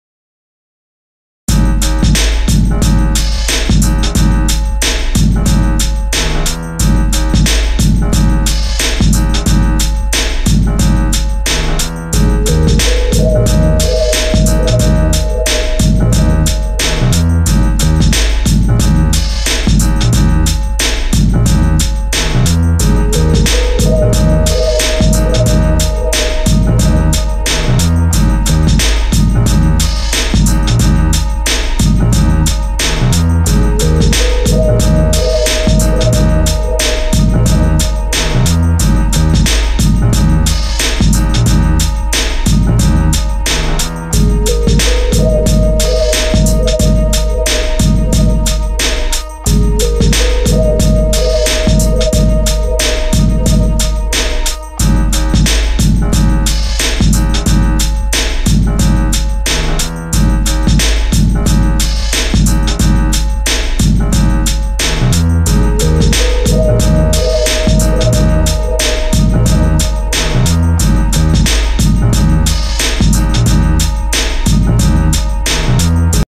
Pindahan kontrakan ke kontrakan di Pasirsari Cikarang
kontrakancikarang Pickup pindahan dari kontrakan ke kontrakan di daerah Pasirsari Cikarang.